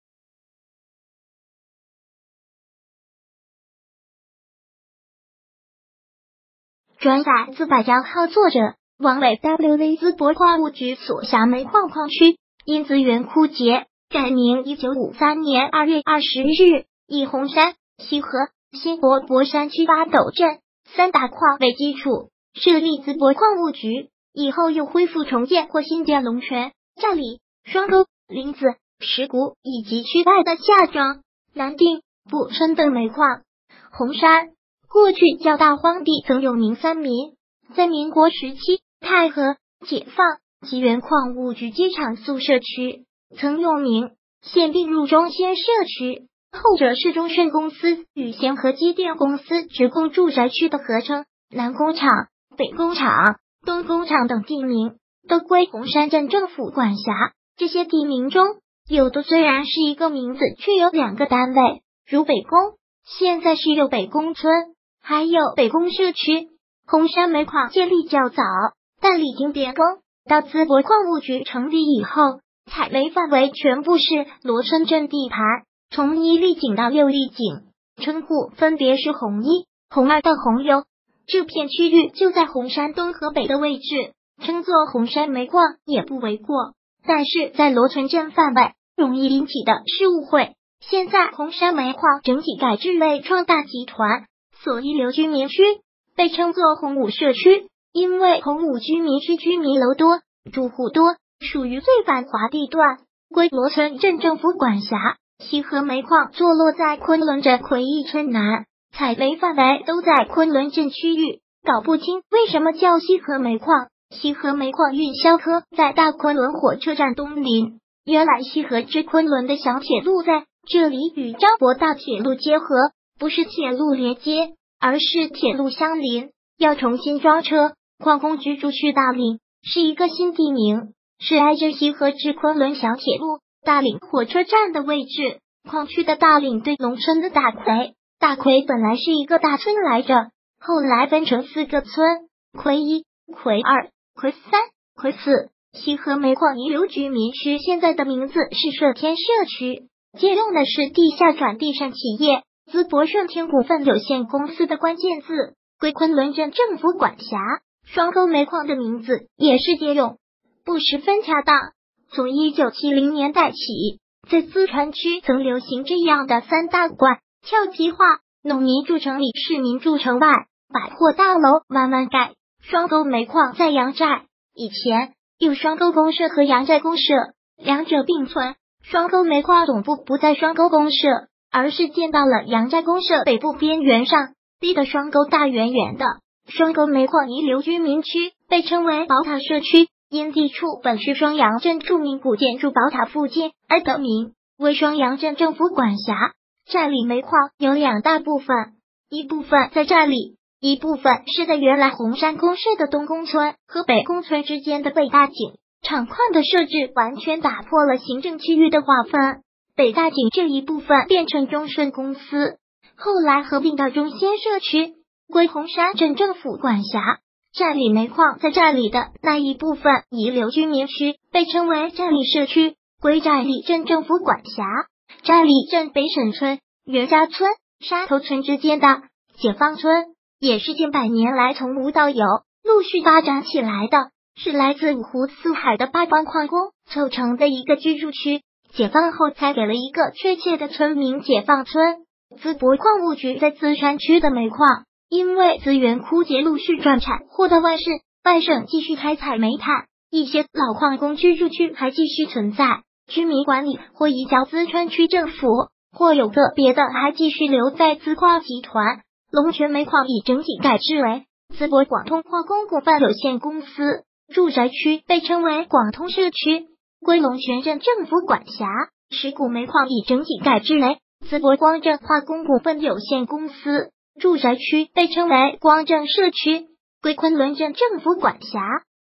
淄博矿务局所辖煤矿矿区因资源枯竭改名
转载自百家号作者：王伟wv淄博矿务局所辖煤矿矿区因资源枯竭改名1953年2月20日，以洪山、西河、新博（博山区八陡镇）三大矿为基础，设立淄博矿务局。以后又恢复重建或新建龙泉、寨里、双沟、岭子、石谷以及区外的夏庄、南定、埠村等煤矿。洪山，过去叫大荒地（曾用名三民，在民国时期）、太和、解放（即原矿务局机厂宿舍区，曾用名，现并入中先社区；后者是中舜公司与先河机电公司职工住宅区的合称）、南工（厂）、北工（厂）、东工（厂）等地名，都归洪山镇政府管辖。这些地名中，有的虽然是一个名字却有两个单位，如北工，现在是有北工村，还有北工社区。洪山煤矿建立较早，但历经变更，到淄博矿务局成立以后，采煤范围全部是罗村镇地盘，从一立井到六立井，称呼分别是洪一、洪二到洪六，这片区域就在“黉山”东和北的位置，称作“洪山煤矿”也不为过，但是在罗村镇范围，容易引起的是误会。现在洪山煤矿整体改制为“创大集团”，所遗留居民区被称作“洪五社区”，因为“洪五”居民区居民楼多、住户多、属于最繁华地段，归罗村镇政府管辖。西河煤矿坐落在昆仑镇奎一村南，采煤范围都在昆仑镇区域，搞不清为什么叫“西河”煤矿？西河煤矿运销科在大昆仑火车站东邻，